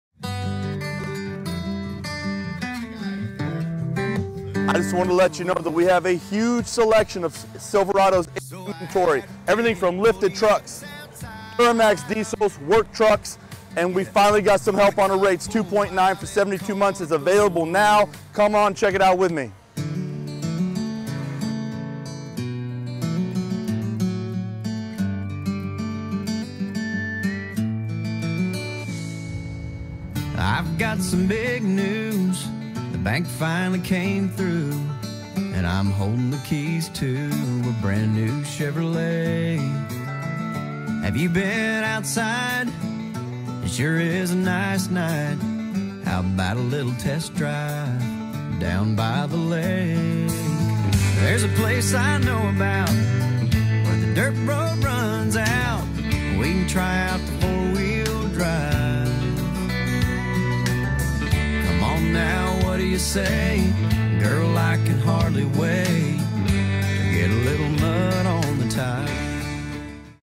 Huge selection of Chevy Silverado trucks.
We have a huge selection of Chevy Silverado truck inventory. Available at 2.9 for 72 months for qualified buyers. Come check it out with Shane!